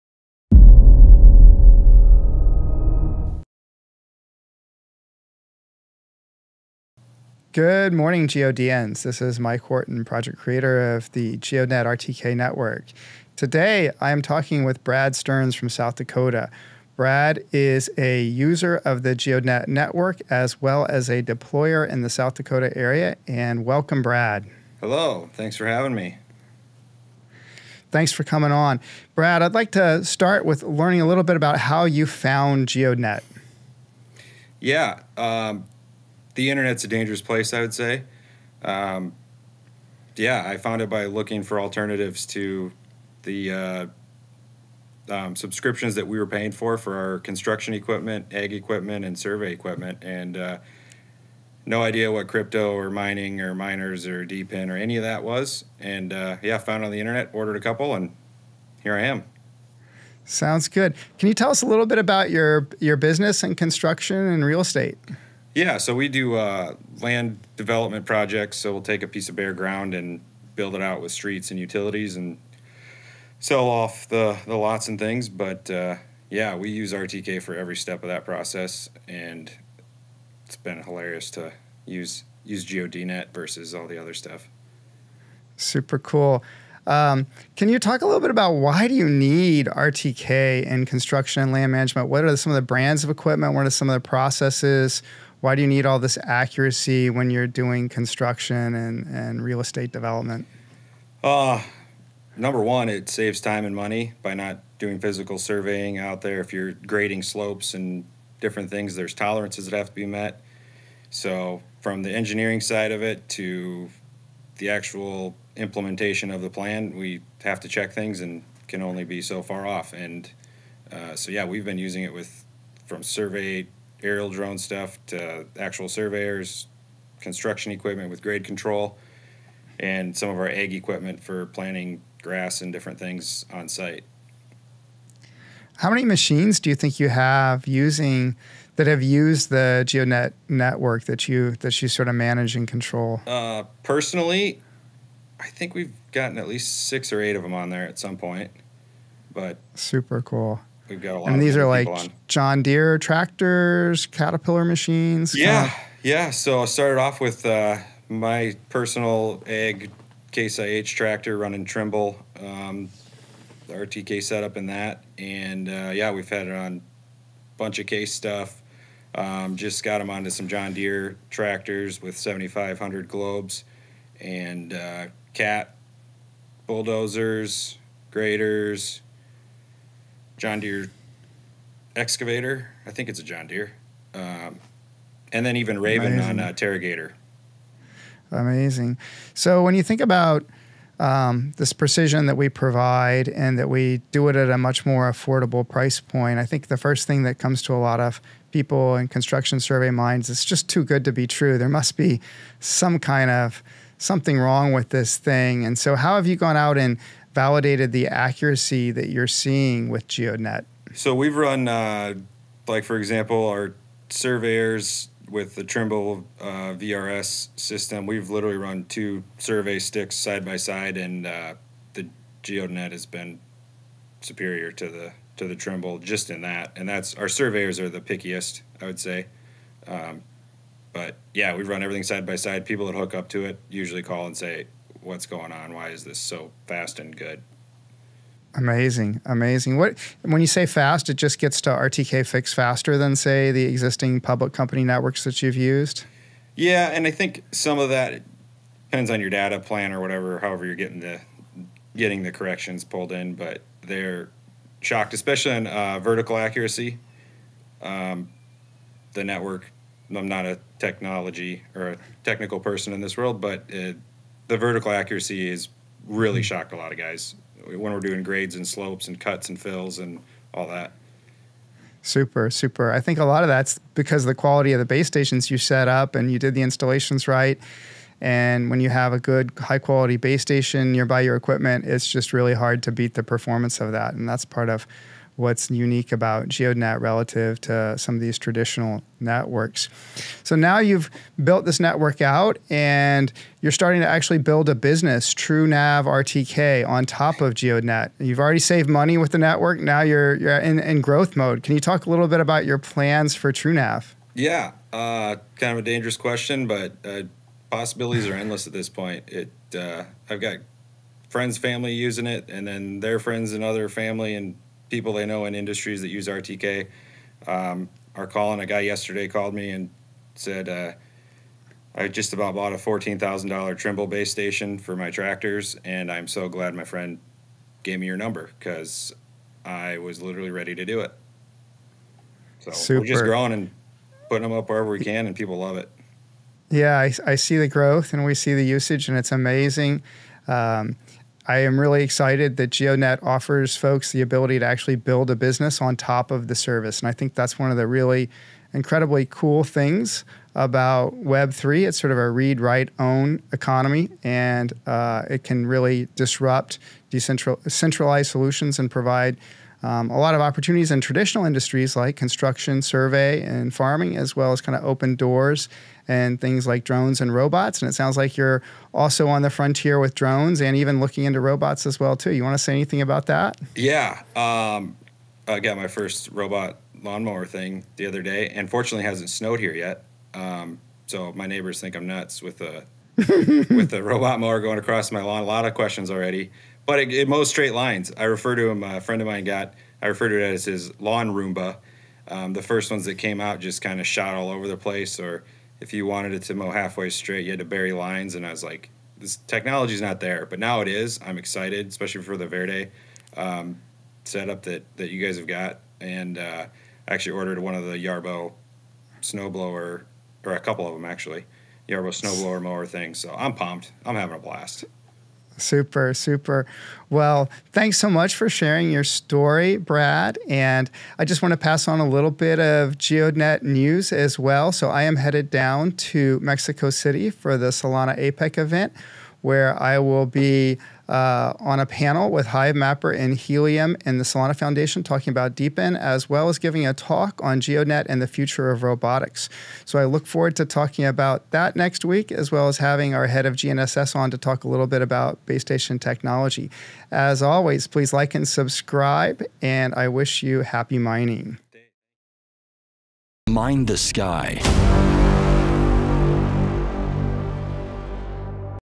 Why Pros Are Switching To GEODNET! | How GEODNET is Revolutionizing Construction & Farming
In this week's podcast, Mike Horton sits down with Brad Stearns from South Dakota, a GEODNET user and deployer. Brad shares how he discovered GEODNET while searching for cost-effective RTK solutions for his construction and land development business, saving time and money compared to traditional subscriptions.

💡 Topics Covered:
✅ How GEODNET outperforms traditional RTK subscriptions
✅ RTK’s role in construction, surveying, and farming
✅ Real-world testing vs. Trimble—GEODNET’s accuracy surprises experts
✅ Expanding RTK networks and building a business with TrueNav RTK
✅ The future of automation—RTK-powered drones & robotic mowers

Brad’s experience proves that affordable, high-precision RTK is here—and it’s disrupting the industry!